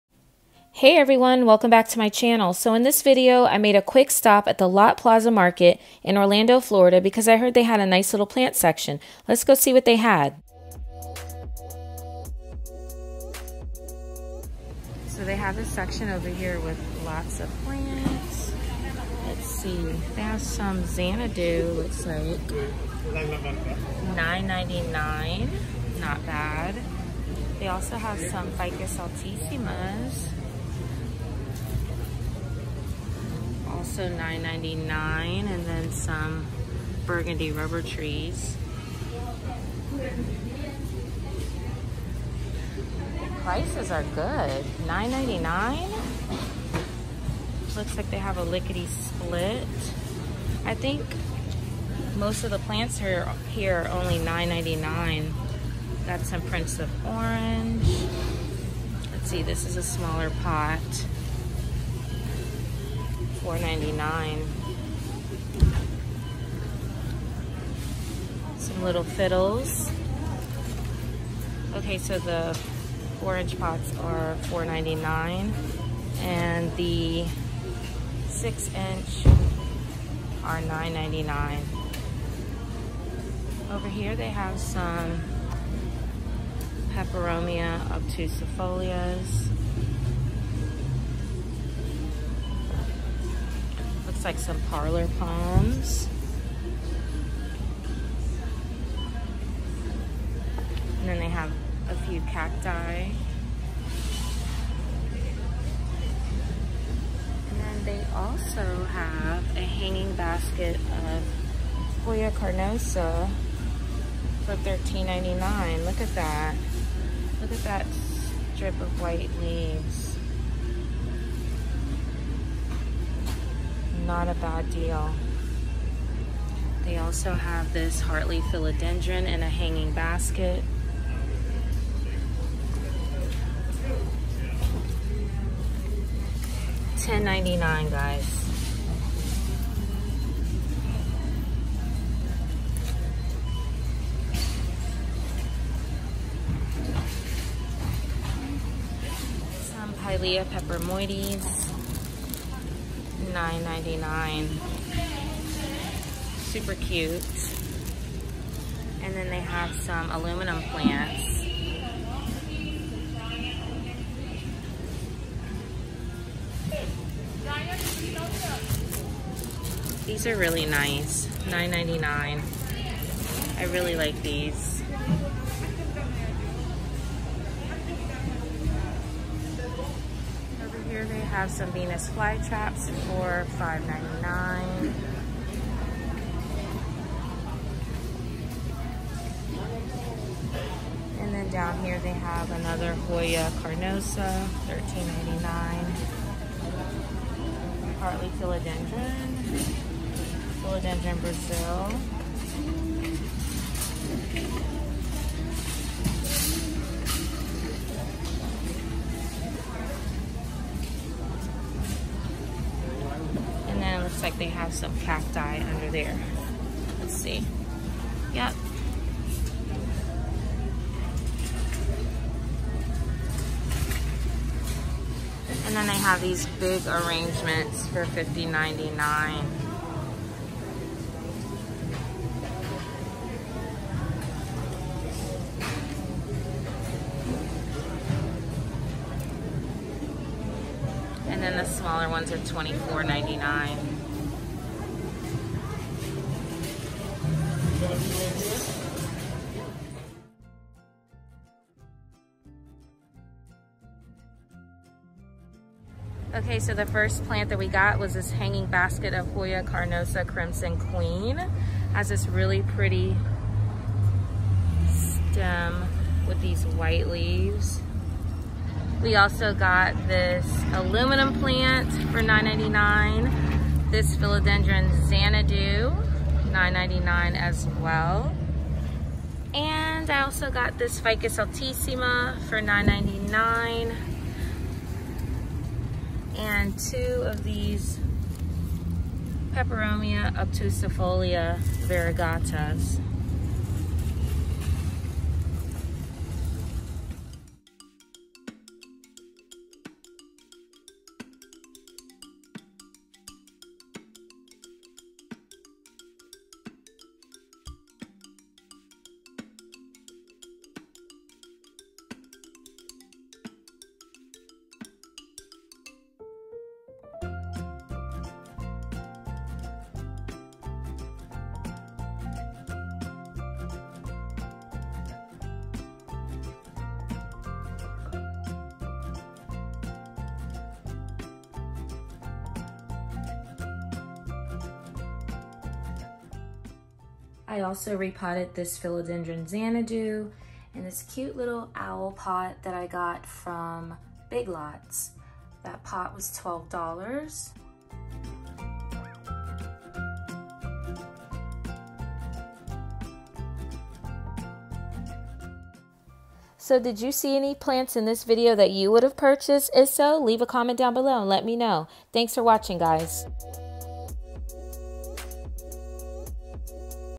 Grocery Store Plant Shopping at Lotte Plaza Market || FICUS ALTISSIMA, PRINCE OF ORANGE & HOYAS
In this video I am grocery store plant shopping at Lotte Plaza Market in Orlando, FL. They had so many cheap plants! Come see what they had!

I also make plant T-shirts! or just click on the right side of my channel banner!

Mailing Address:
Island Girl Creations

💚 Connie 💚